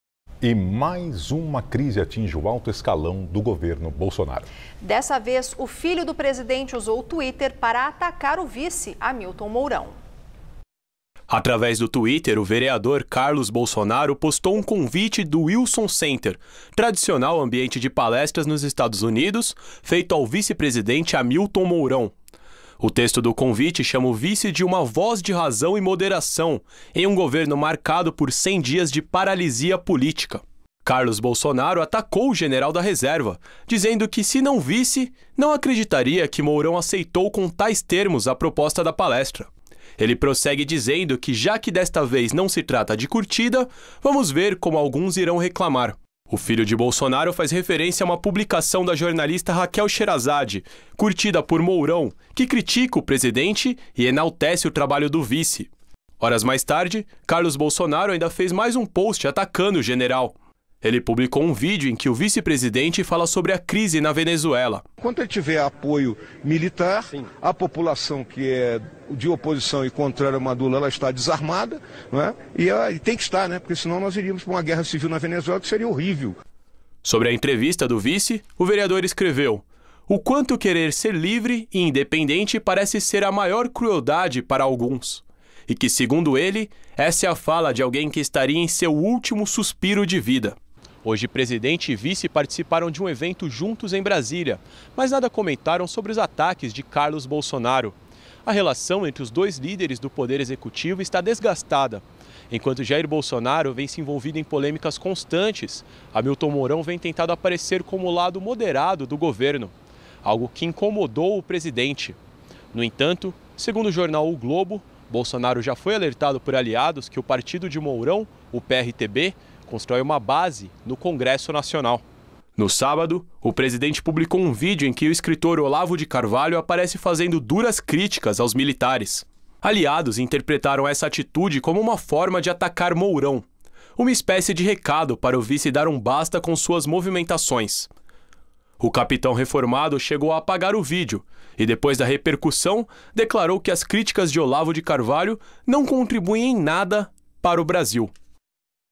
Filho de Bolsonaro ataca o vice Hamilton Mourão
Mais uma crise atinge o alto escalão do governo Bolsonaro.
Dessa vez, o filho do presidente usou o twitter para atacar o vice, Hamilton Mourão.